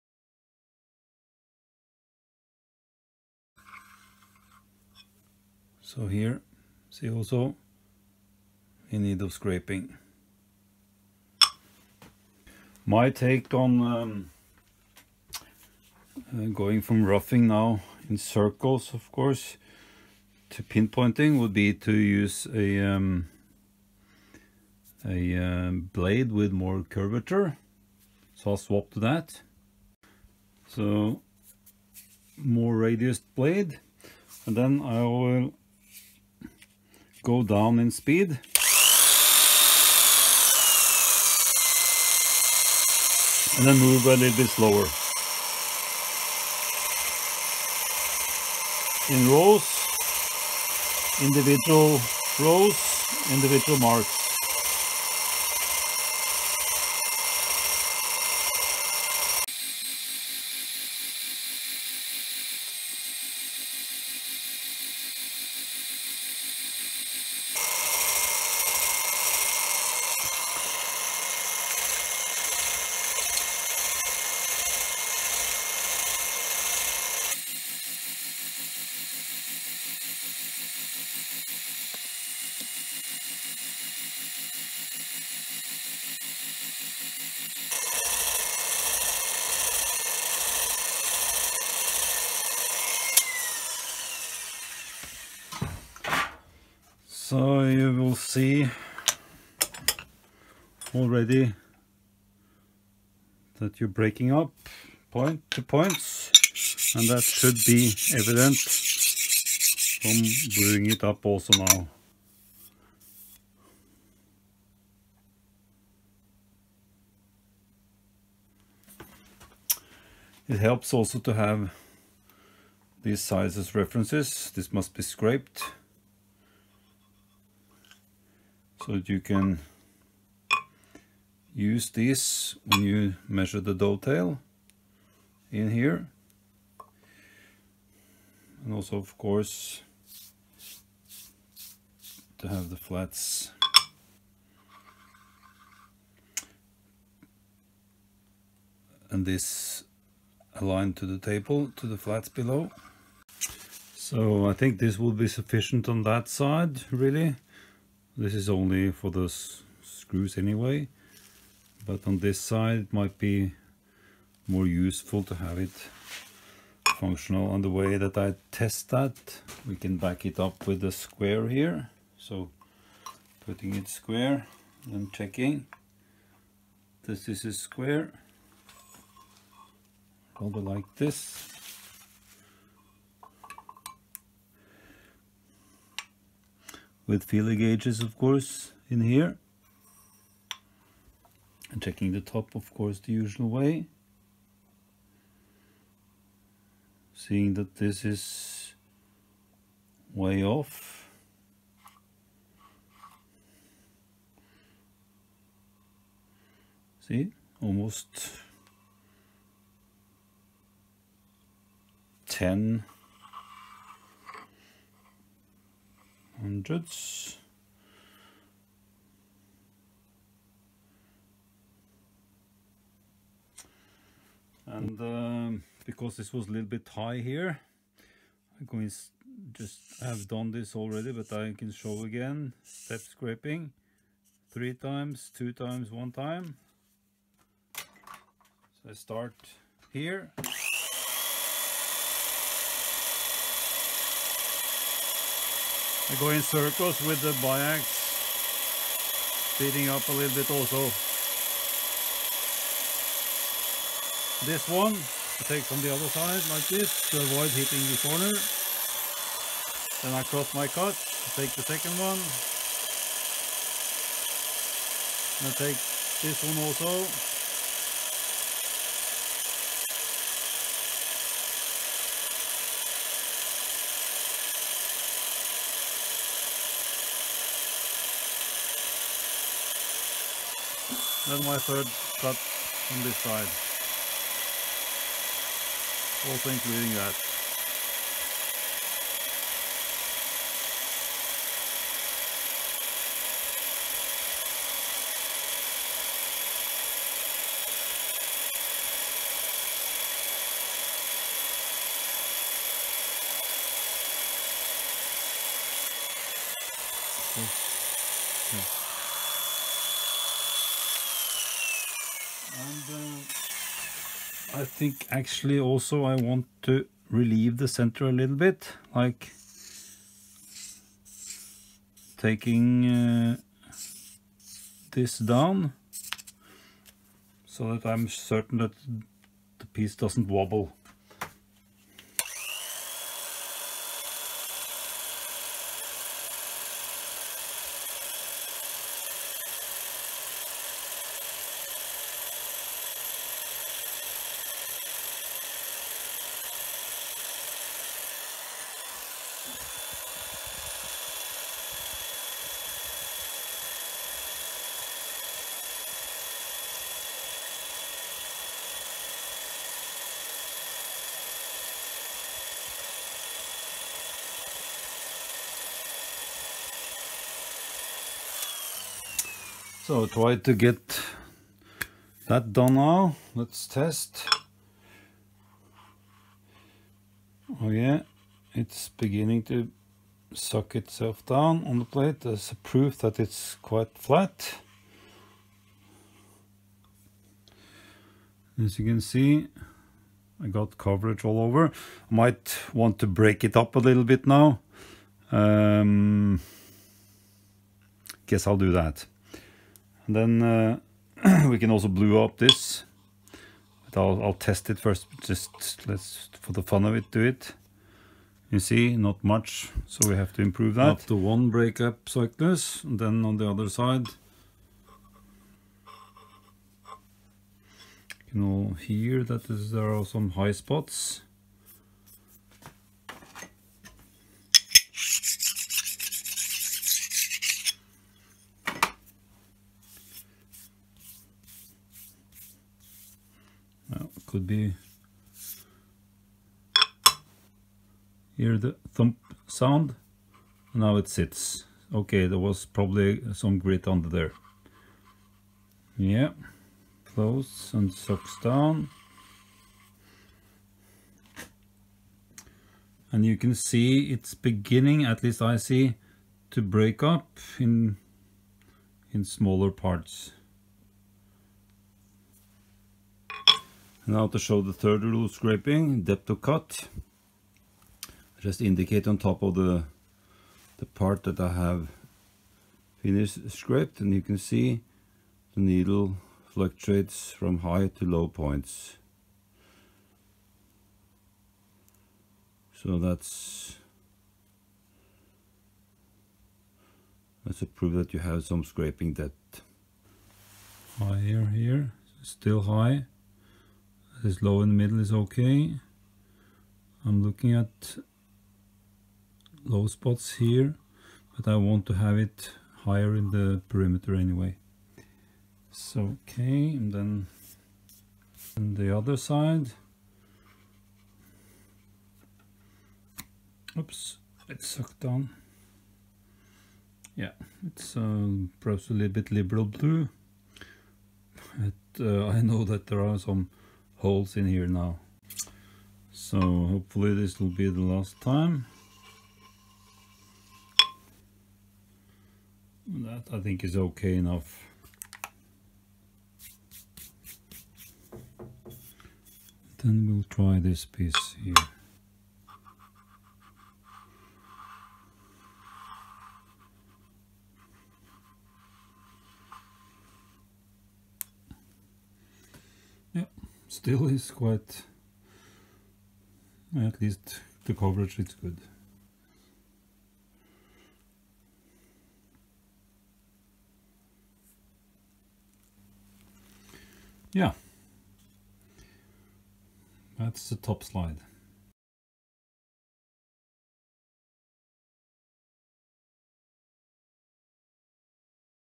Myford 7x7 top slide and base 1
Rules of scraping: individual marks, individual rows, depth of cut, hinge the part
Use of BIAX machine scraper: circles technique, speed for roughing/finishing, radius of blades, avoid rounding edges, step scraping, relieving center
alignment of parts, testing vs plate, reference surfaces, plate cleanliness.

I will also take the opportunity to suggest that if yo  want to learn scraping, you can save yourself a ton of time if you take a scraping class. I have been lucky to be able to have a professional to teach me and I would heartily recommend the same. You can google BIAX or scraping and then find online material as well as enlist to classes